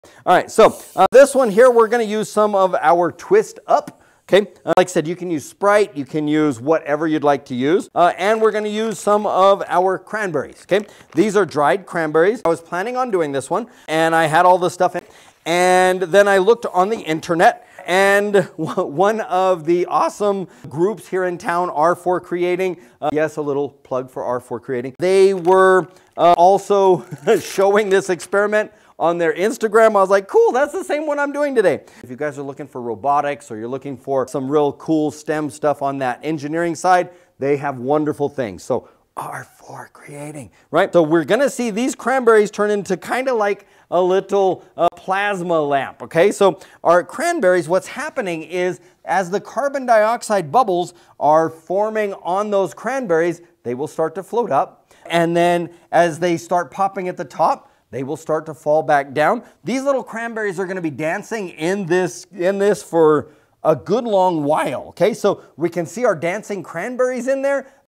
Cranberries Dance: A Fun STEM Experiment!🕺🍒
Watch as we explore a fascinating experiment with cranberries that creates a mesmerizing dance effect! As bubbles form and pop, our cranberries rise and fall like a plasma lamp. Join us for this exciting journey into STEM!